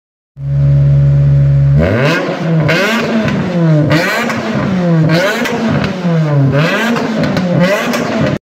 Ferrari F430 Replica kit car Exhaust note
Hear the exhaust sound of a Ferrari F430 kit car replica based on a Toyota Celica vvtli built not bought